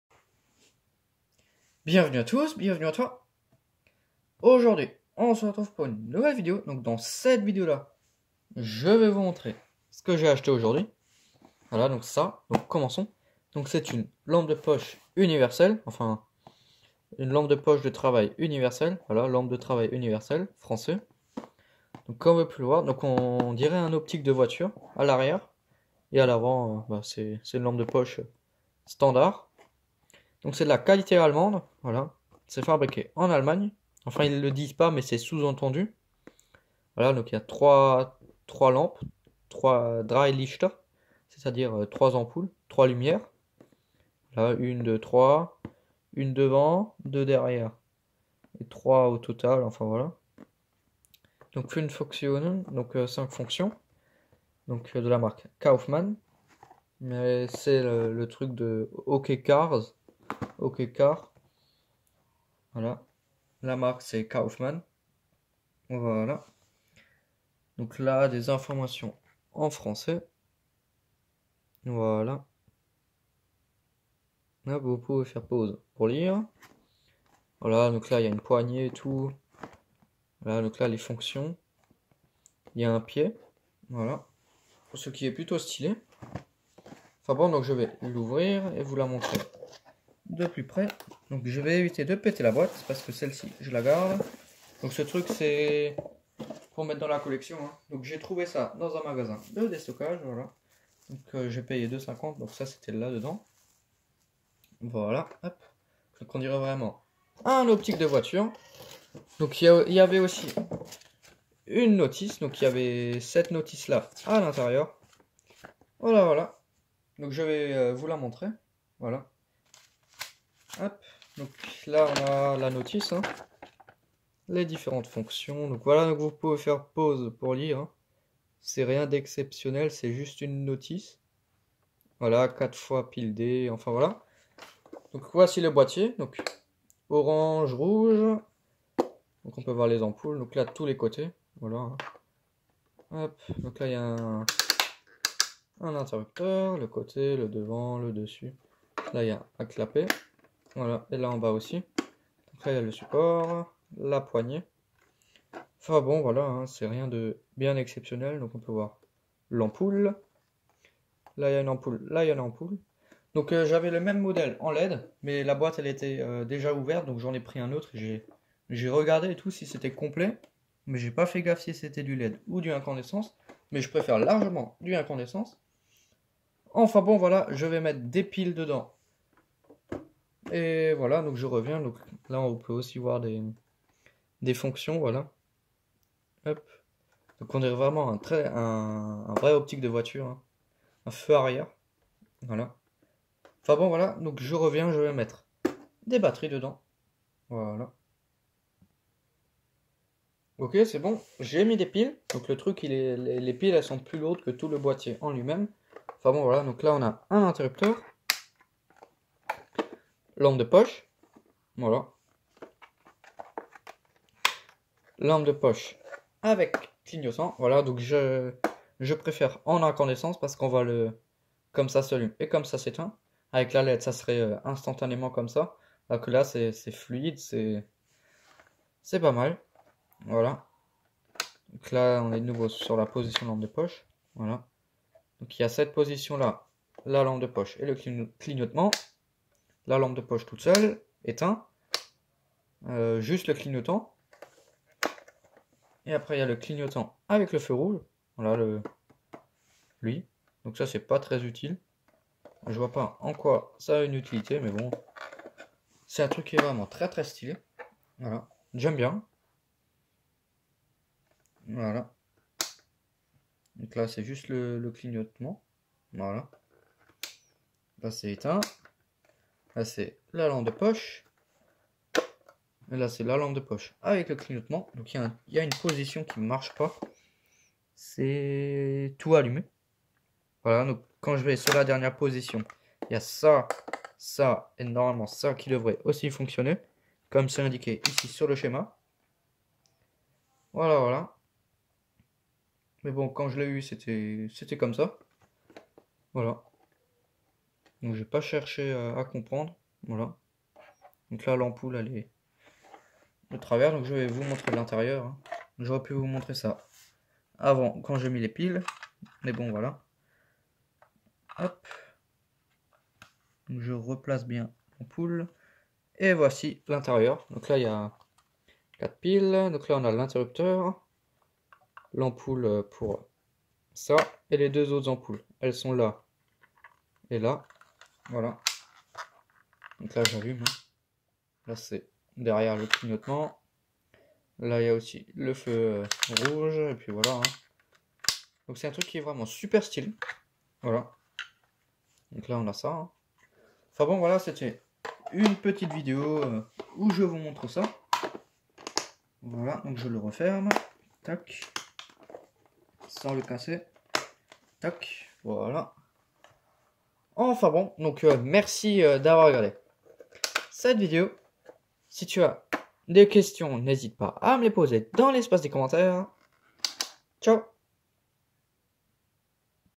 Lampe de poche spécial / présentation+test
Bienvenue à toi ! :D

Aujourd'hui, présentation d'une lampe de poche plutôt cool ;)

Oubli pas de liker et à tabonner ça fait toujours plaisir ^^